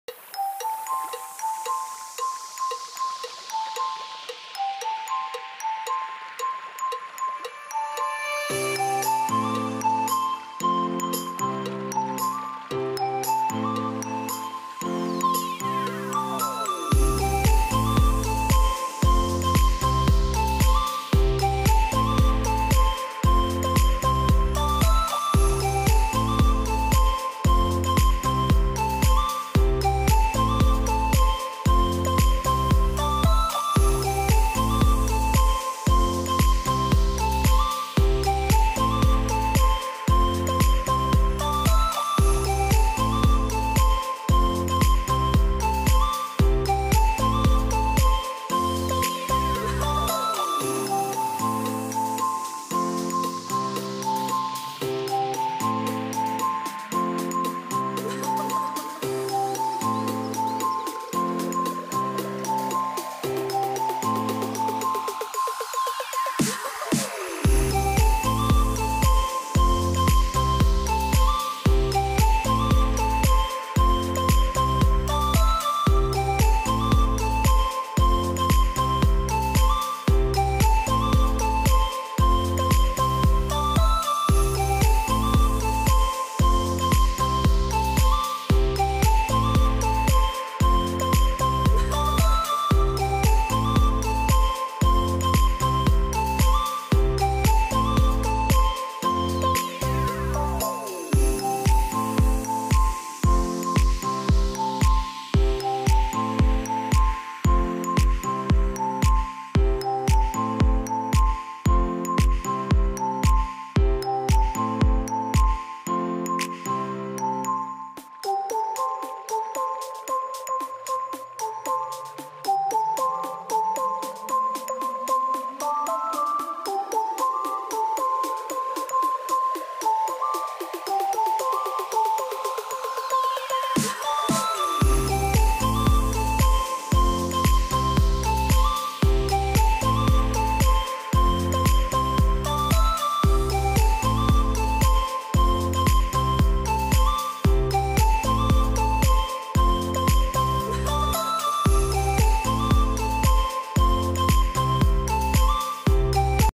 Bracelet Making Process By Using Beads..
Namaste
                  Aj ke es video ap dekheinge ki kaise beads ki madad se sunder sa bracelet banate hain.
Background Music Detail